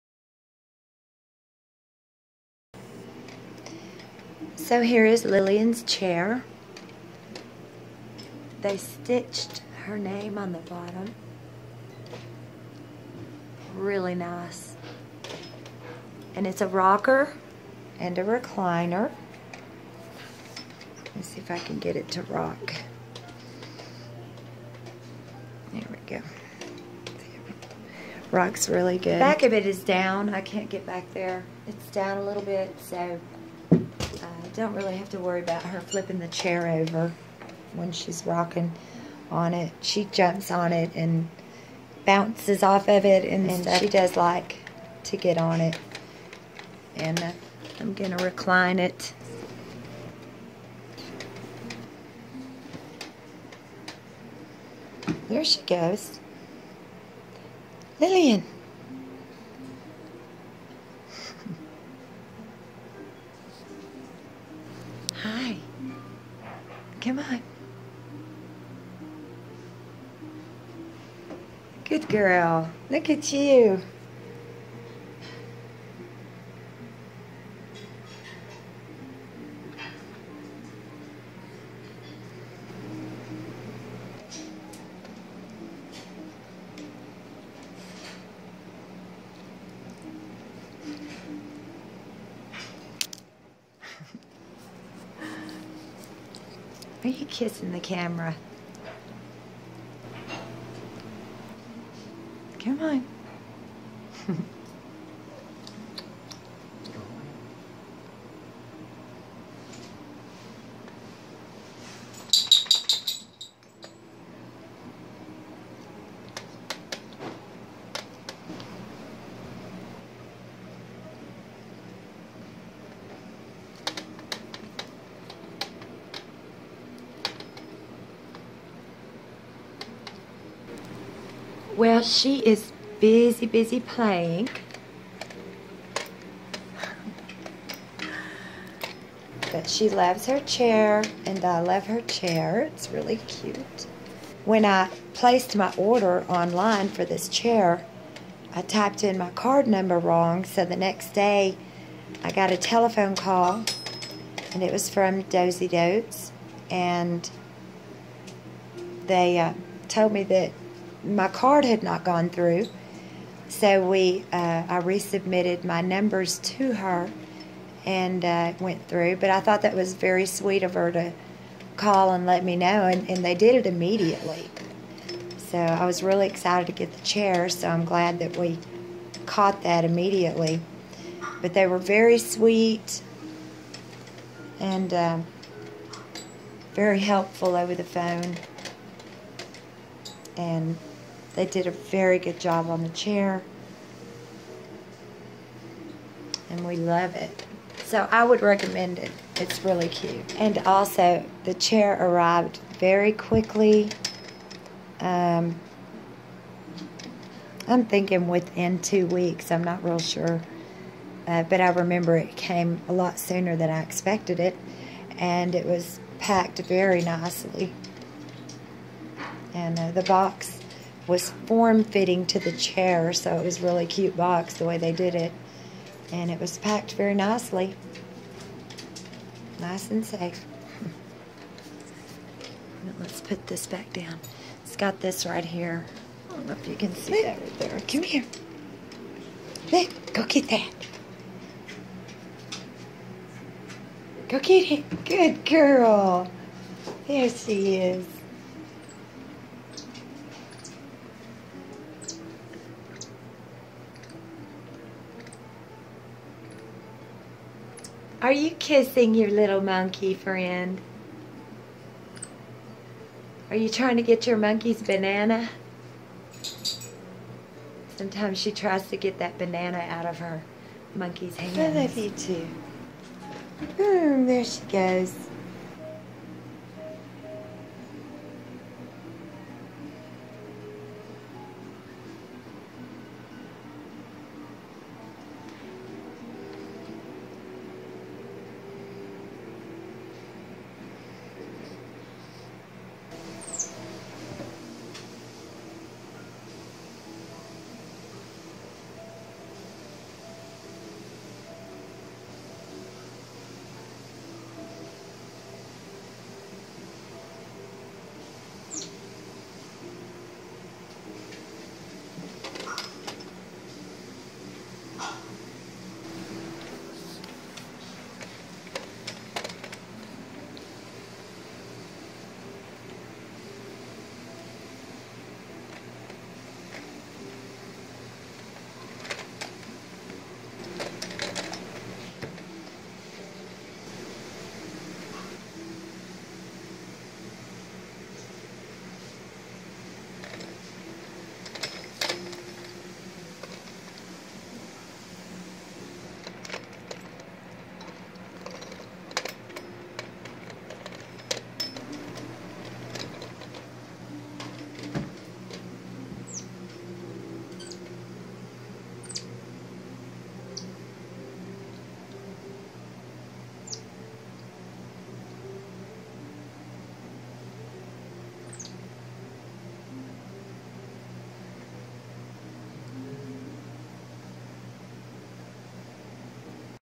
Pet Monkey Recliner
LL (#22) Pet Monkey Recliner
Dozydotes Rocker Chair for Baby Monkey  Lillian loves jumping and playing on her new rocker recliner chair from Dozydotes.  This chair is very cute and substantial.  Although it doesn't fully recline it is perfect for a small child or monkey :)  It is very soft and cushiony and has Lillian's name stitched on the foot rest at no extra cost.  The delivery was in a reasonable amount of time and the packaging was nice keeping the chair safe and secure. The customer service department contacted me when they discovered a problem with my online order (which was completely my fault) and helped me place my order. They were very friendly and made sure I was happy with my order.  Great chair for a really good price, great service, great delivery.  Check it out.  If you enjoy Lillian's video don't forget to give her a big thumbs up and drop her a comment.